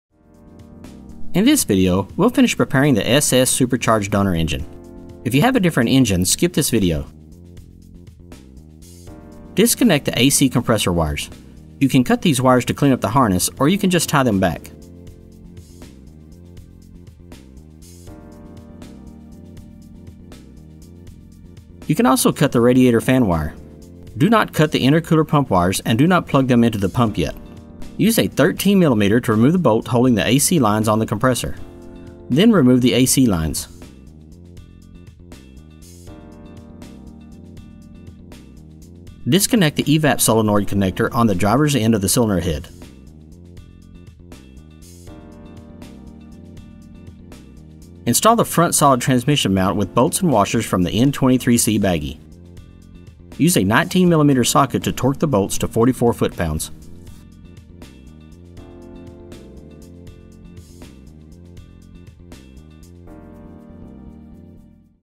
DF Goblin Assembly - SS/SC Engine Preparation
In this video we will finish preparing the SS/SC engine. Only use this video if you are using an SS/SC donor. Other donor-specific videos will be available soon.

Head to our website for more information about the DF Goblin kit car